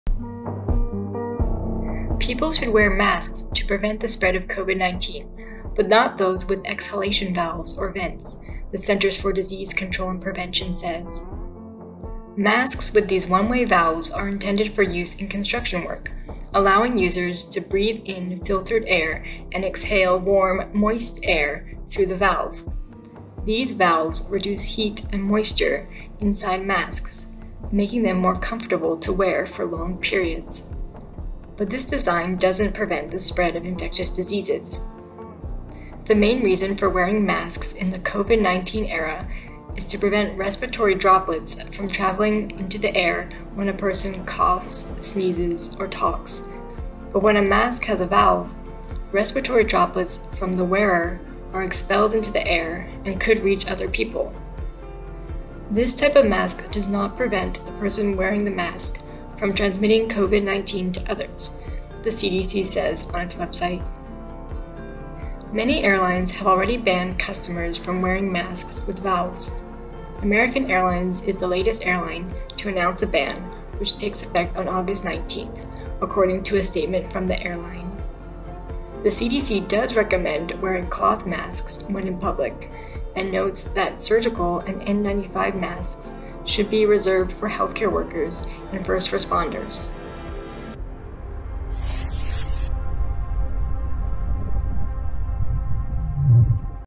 Masks With Valves Don't Stop Virus
Masks with these one-way valves reduce heat and moisture inside masks, making them more comfortable to wear for long periods, but this design doesn't prevent the spread of infectious diseases. Read more about the new CDC recommendation